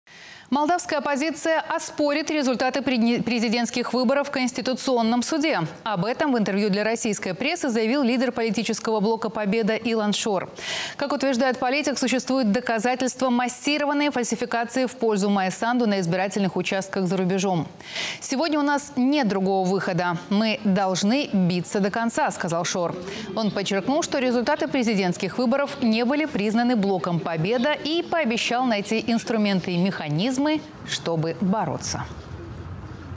Оппозиция оспорит результаты выборов
Молдавская оппозиция оспорит результаты президентских выборов в Конституционном суде. Об этом в интервью для российской прессы заявил лидер политического блока «Победа» Илан Шор.